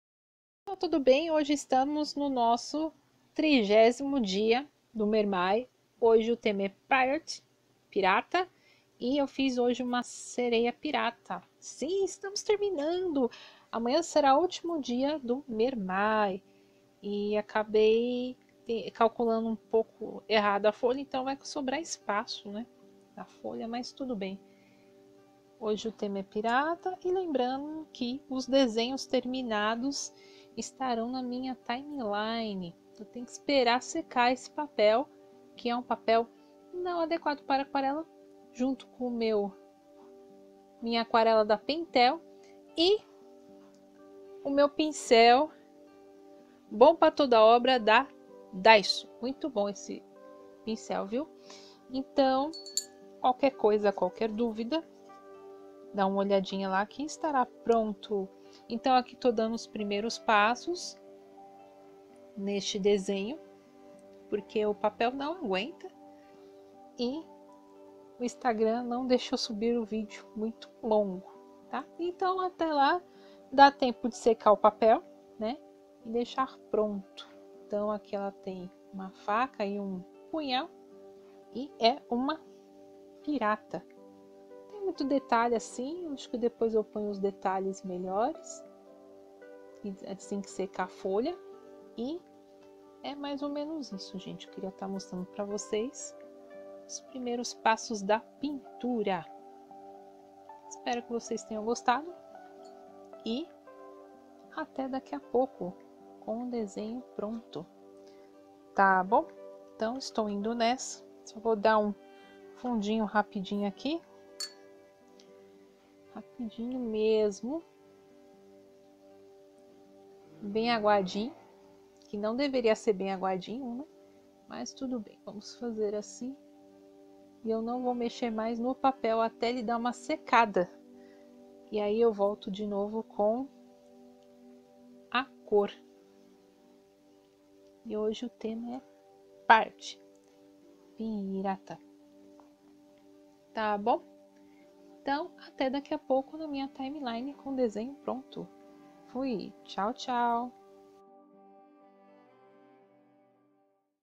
aquarela mermay dia 30 pirate
Olá Amigunh@s! Quem me acompanha no Instagram já deve ter visto alguns desses vídeos de desafios de fazer um desenho por dia, ano passado fiz com aquarela, e portanto como é um vídeo simples de mostrar um dos meus processos de aquarela, nesse caso mais simples ,quis dividir também no Youtube.
O legal de fazer esses desenhos é poder experimentar os resultados, não nos preocupamos se "sai errado" ou não, o importante é o momento e se deixar levar, não é necessário fazer algo muito grande, para estudos é um ótimo aprendizado.